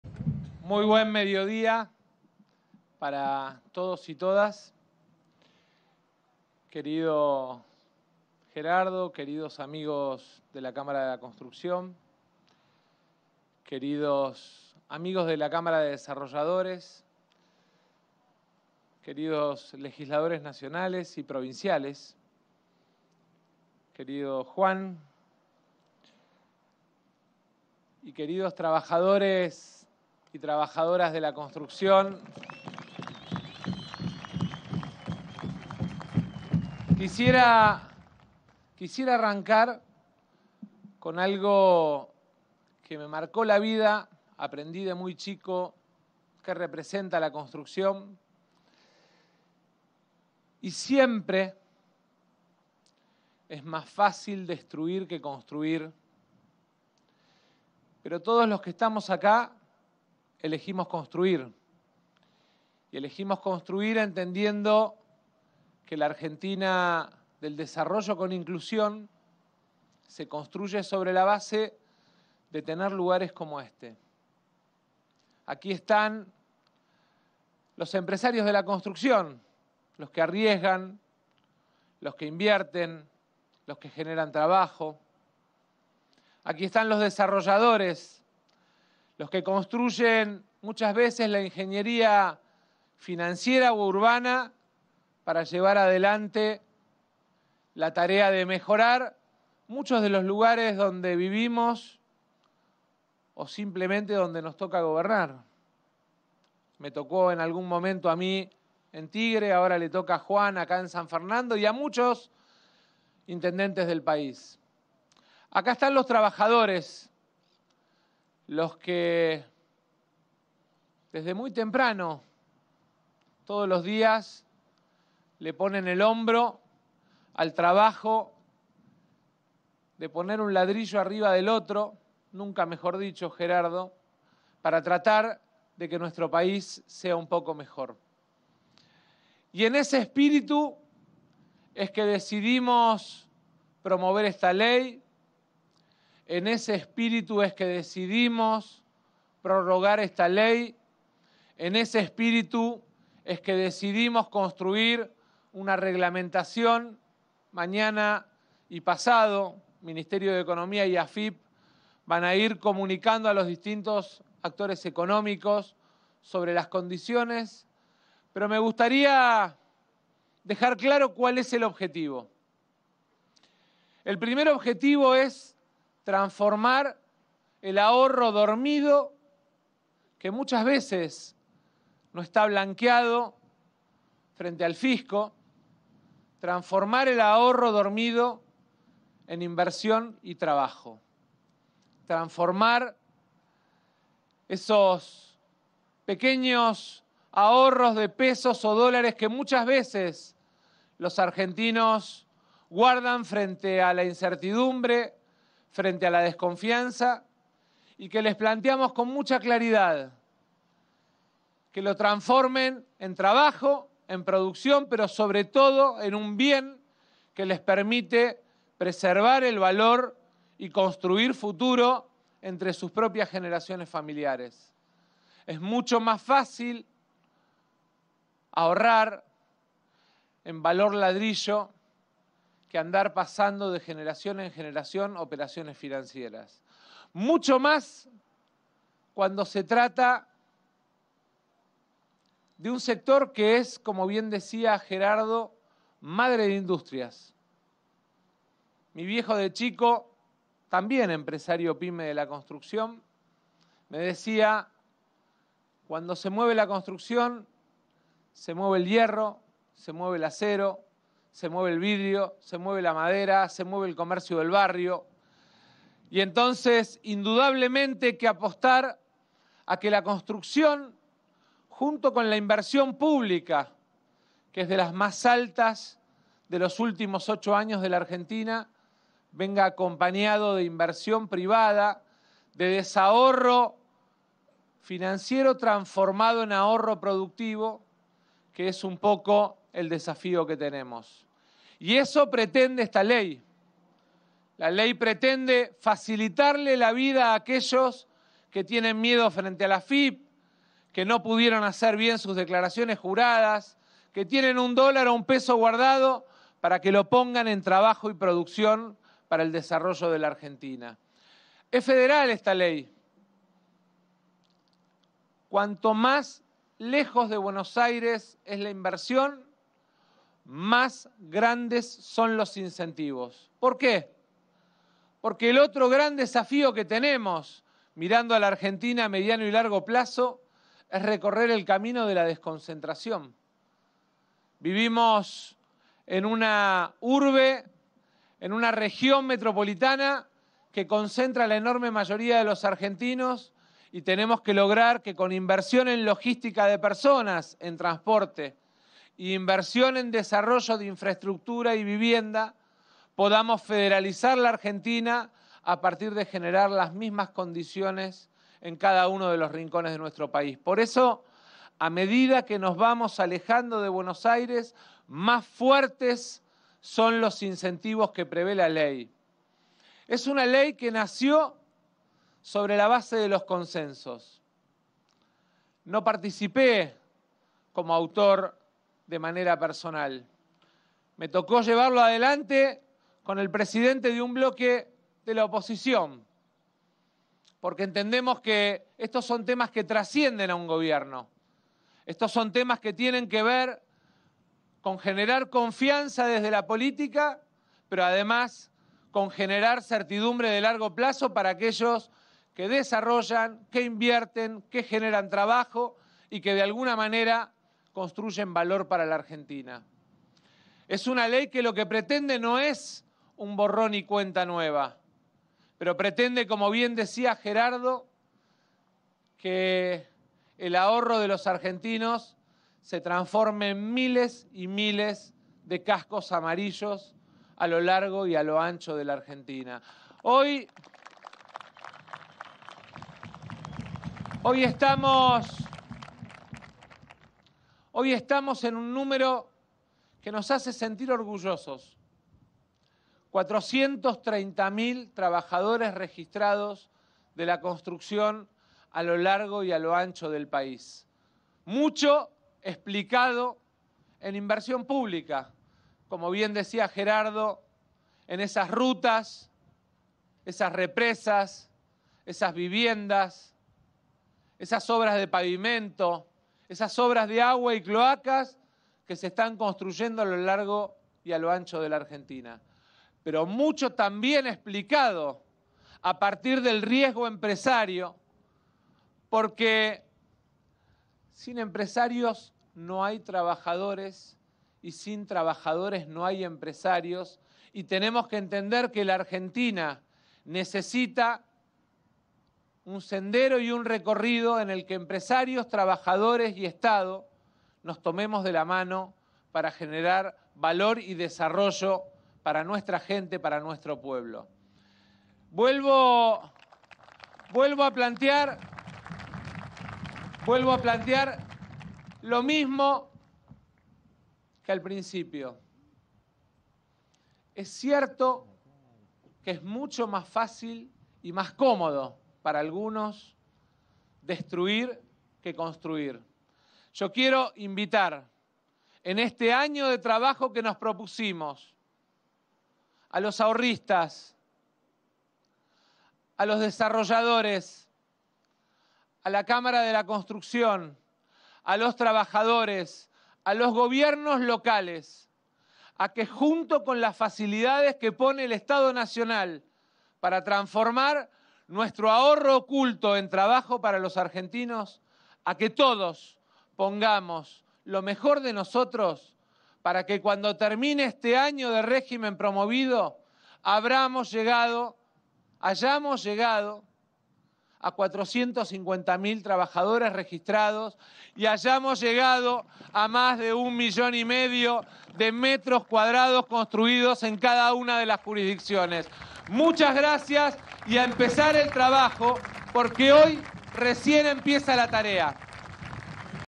22-8-22 | ALIVIO FISCAL PARA LA CONSTRUCCIÓN | ▶️ DISCURSO
Junto al intendente Juan Andreotti, el secretario general de UOCRA, Gerardo Martínez; trabajadores, inversores y Cámaras empresariales, anunciamos la promulgación de la Ley de Incentivo a la Construcción Federal Argentina y Acceso a la Vivienda que genera un alivio fiscal en el sector.

Una Ley que pretende que el ahorro de los argentinos y argentinas se transforme en miles y miles de cascos amarillos a lo largo y ancho del país, porque tiene un fuerte carácter federal

Es mucho más fácil destruir que construir. Nosotros elegimos construir.
Por eso los invitamos a utilizar esta herramienta fiscal para multiplicar el trabajo y el crecimiento nacional.